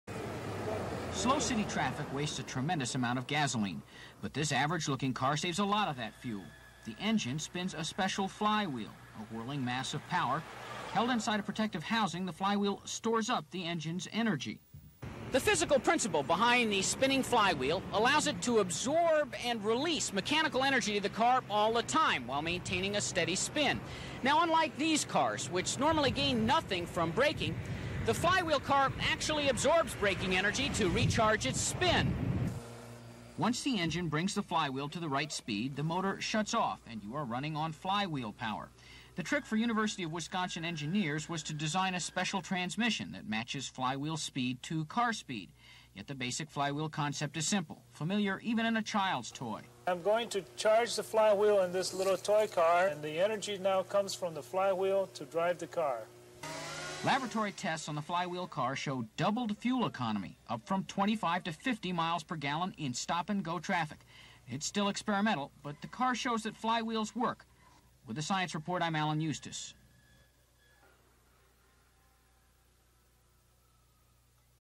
Experimental Spinning Flywheel Car Demonstration (July 2, 1980)
Report on the use of a spinning flywheel to make a more efficient transmission and use less gas. Footage of a flywheel-powered car being driven and tested in a garage by a scientist; a flywheel attached to a transmission under a car’s hood. Interview and demonstration of a toy flywheel car by a University of Minnesota scientist. Natural sound and VoiceOver.

Reporter: Eustis, Allan

A flywheel is a mechanical device which uses the conservation of angular momentum to store rotational energy; a form of kinetic energy proportional to the product of its moment of inertia and the square of its rotational speed. In particular, assuming the flywheel's moment of inertia is constant (i.e., a flywheel with fixed mass and second moment of area revolving about some fixed axis) then the stored (rotational) energy is directly associated with the square of its rotational speed.

Since a flywheel serves to store mechanical energy for later use, it is natural to consider it as a kinetic energy analogue of an electrical inductor. Once suitably abstracted, this shared principle of energy storage is described in the generalized concept of an accumulator. As with other types of accumulators, a flywheel inherently smooths sufficiently small deviations in the power output of a system, thereby effectively playing the role of a low-pass filter with respect to the mechanical velocity (angular, or otherwise) of the system. More precisely, a flywheel's stored energy will donate a surge in power output upon a drop in power input and will conversely absorb any excess power input (system-generated power) in the form of rotational energy.

Common uses of a flywheel include smoothing a power output in reciprocating engines, energy storage, delivering energy at higher rates than the source, controlling the orientation of a mechanical system using gyroscope and reaction wheel, etc. Flywheels are typically made of steel and rotate on conventional bearings; these are generally limited to a maximum revolution rate of a few thousand RPM.[1] High energy density flywheels can be made of carbon fiber composites and employ magnetic bearings, enabling them to revolve at speeds up to 60,000 RPM (1 kHz).[2]